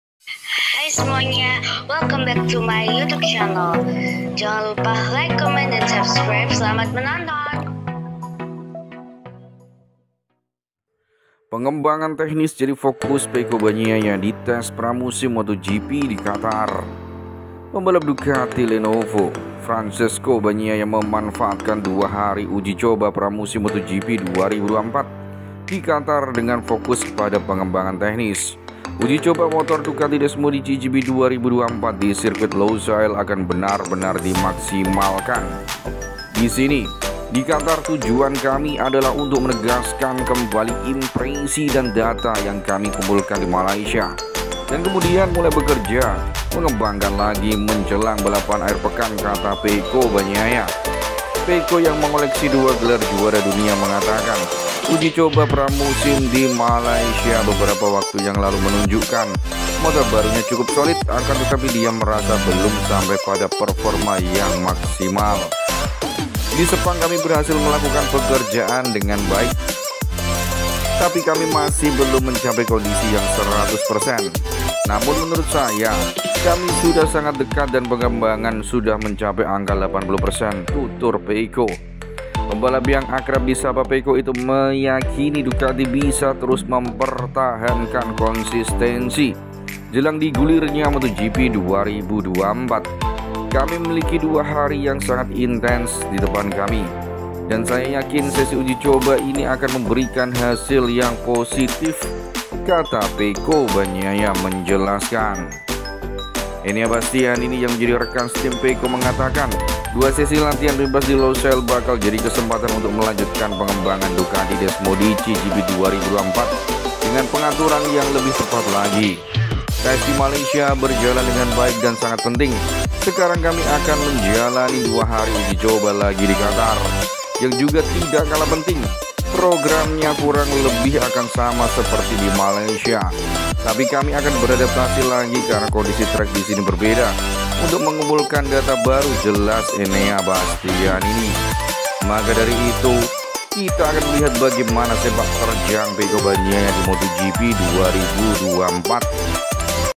Test Qatar 2024: Pecco Tercepat? Pecco Bagnaia fokus pada pengembangan teknis di tes pramusim Qatar!
Pembalap Ducati Lenovo Francesco Bagnaia mengatakan akan memanfaatkan dua hari uji coba pramusim MotoGP 2024 terakhir di Sirkuit Internasional Lusail, Qatar, dengan fokus pada pengembangan teknis motor Desmosedici GP 2024.